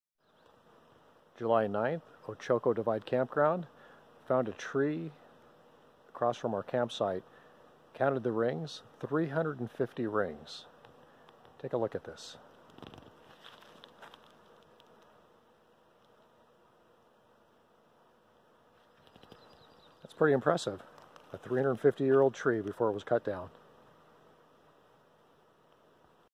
2019 July 9 - Ochoco Divide Campground - 350 Year Old Tree
Counted growth rings on a fallen tree.